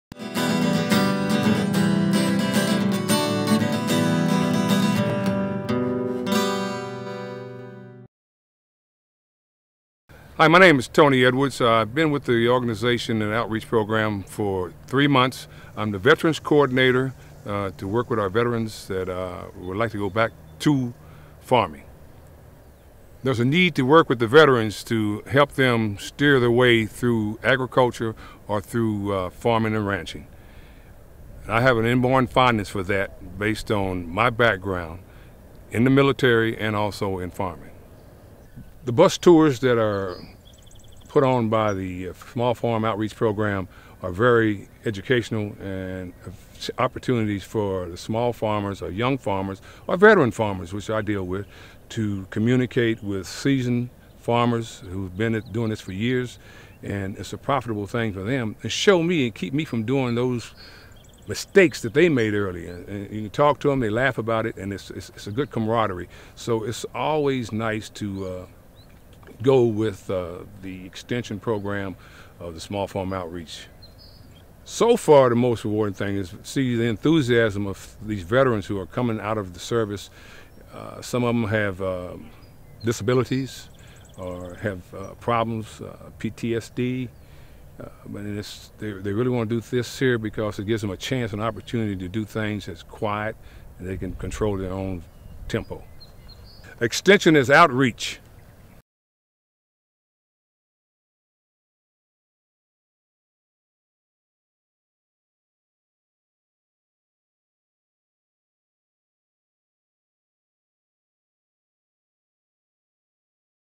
Tony Edwards - Small Farm Outreach Agent - VSU
Meet Tony Edwards a Small Farm Outreach agent that helps veterans become successful farmers.